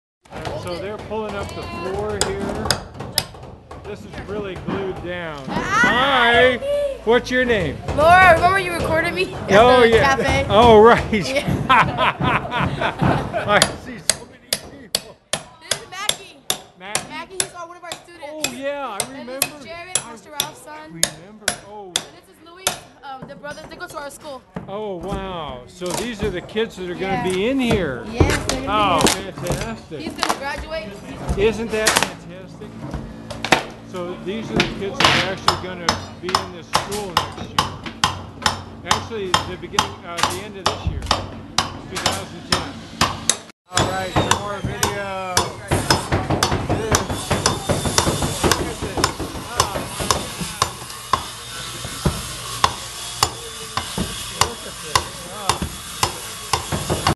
SFCA BUILD IT 10.01.16 Sat City Academy Kids & Laura
Some of the students from City Impacts San Francisco City Academy spend their day off on Saturday helping building their new school!!!  WOW!!!